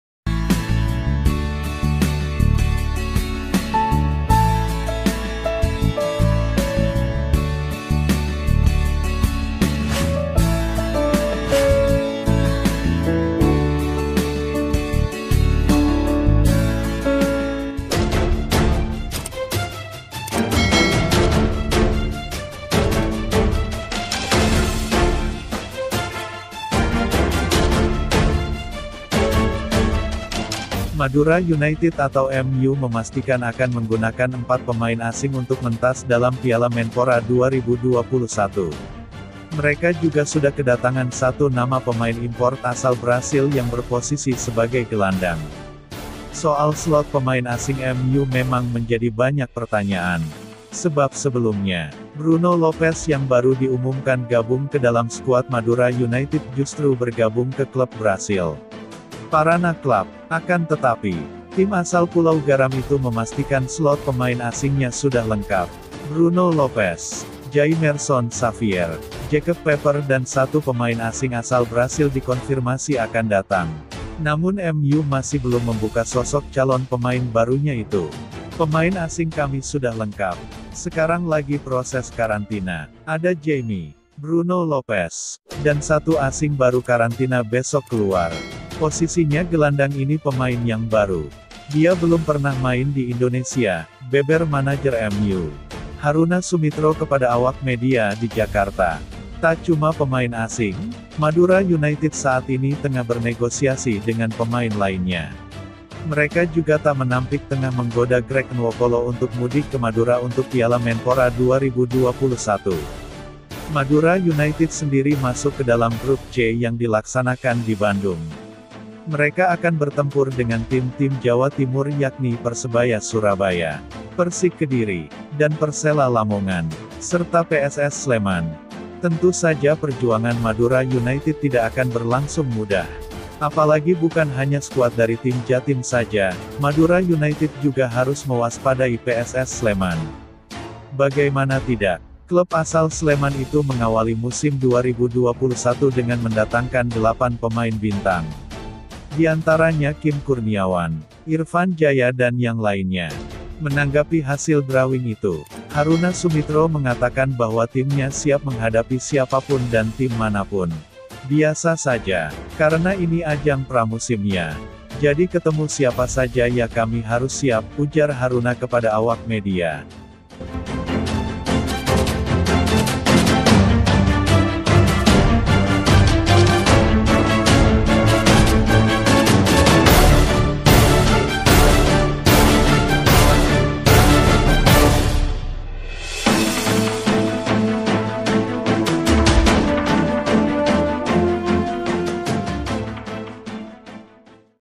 BERITA TERBARU HARI INI ~ 10/3/2021 MADURA UNITED SIAP BERLAGA DI PIALA MENPORA ~ BERITA TERKINI
Chanel ini memberikan berita nasional yang di rangkum singkat,semoga menambah wawasan kita.